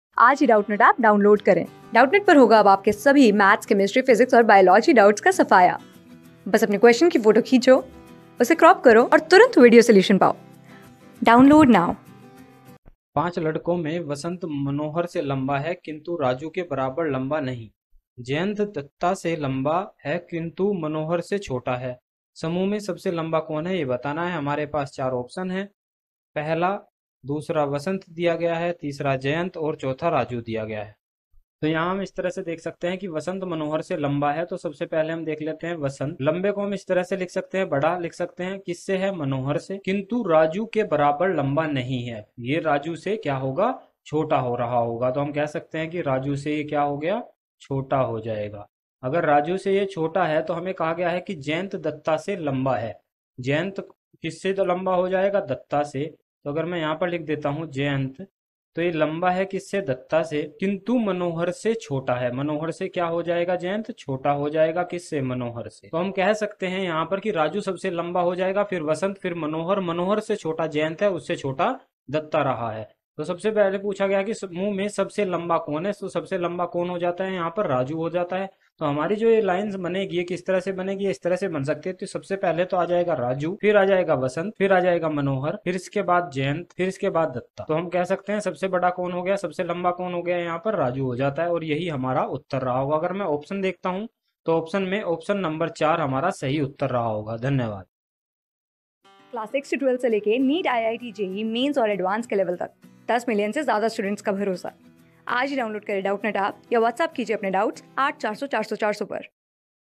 पाँच लड़को में , वसंत , मनोहर से लम्बा है किन्तु राजूके बराबर लम्बा नहीं | जयंत , दत्ता से लम्बा ...
पाँच लड़को में , वसंत , मनोहर से लम्बा है किन्तु राजूके बराबर लम्बा नहीं | जयंत , दत्ता से लम्बा है किन्तु मनोहर से छोटा है | समूह में सबसे लम्बा कौन है ?
Class: 14
Subject: REASONING
Chapter: पदानुक्रम /व्यवस्थीकरण
Board:GOVT

👉 Have Any Query? Ask Us.